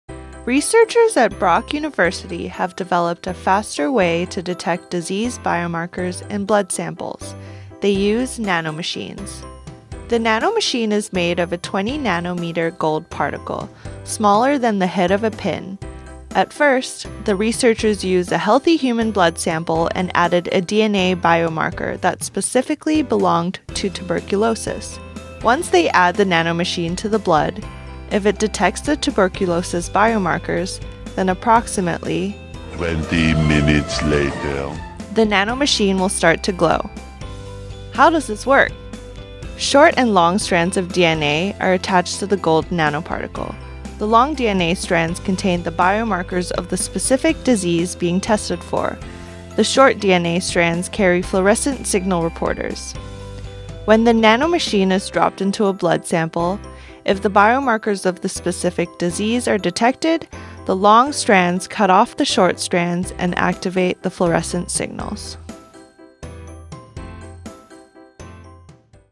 Nanomachines to the Rescue
Researchers at Brock University have developed a faster way to detect disease biomarkers in blood samples — they use nanomachines.